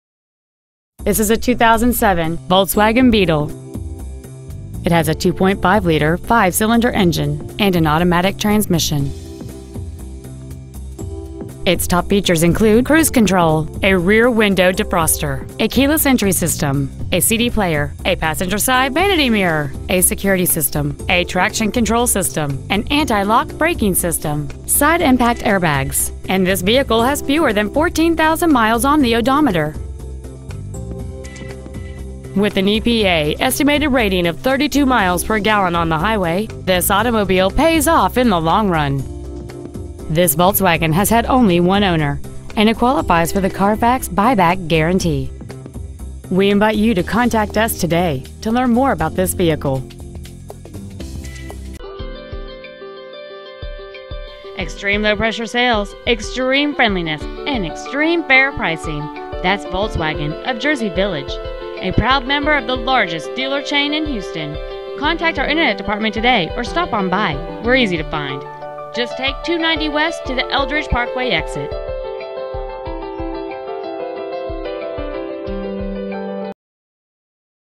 2007 Volkswagen New Beetle Houston TX 77065
Year: 2007
 Make: Volkswagen
 Model: New Beetle
 Engine: 2.5L I-5 cyl
 Trans.: Automatic

 Miles: 13,634

 2007 Volkswagen New Beetle Houston TX